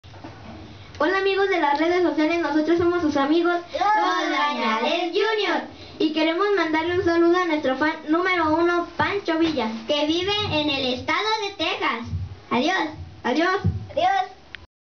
SALUDOS A NUESTRO FAN PANCHO VILLA DE TEXAS "LOS BAÑALES JUNIORS"
Los Bañales Juniors saludando a nuestro fan numero 1
Pancho Villa de Texas, gracias por su apoyo compadrito
saludos!

"CAMISAS RANGER´S", "RESISTOL y STETSON HATS MEXICO" Y "EL POTRO NORTEÑO DE NUESTRA MADRINA ALMA SANTANA" Patrocinadores Oficiales DE LOS BAÑALES JUNIOR